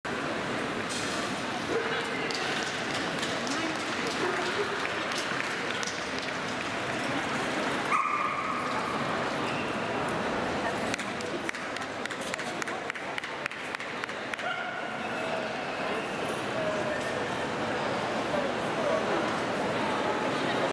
CACIB Bremen Internationale 1.8.2015 - Lou Lou und Camina BOB - Chart Polski
7. Internationale Hundeausstellung 1.8.2015 in Bremen  "Bremen bellt und schnurrt"
Richter Herr  Tomasz Kuszyk PL
"is barking Bremen and purrs" 7th International Dog Show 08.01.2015 in Bremen
Judge Mr. Tomasz Kuszyk PL